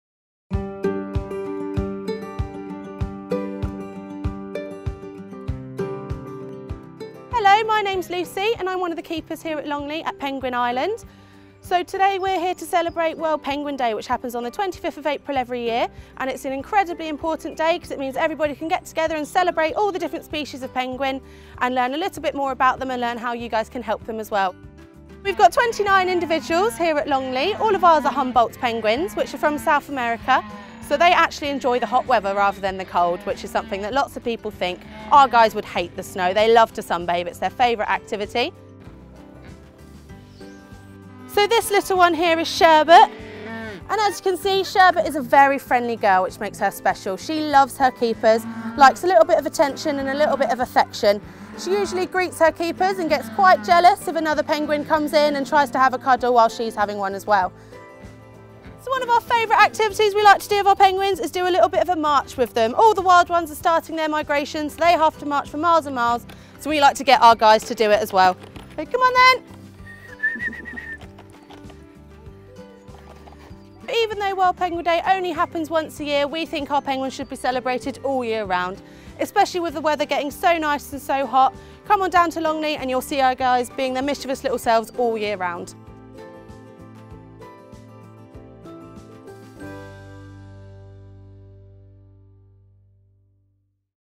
World Penguin Day
We're celebrating ‪‎World Penguin Day‬ with our Humboldt penguins with a special day of activities down at Penguin Island.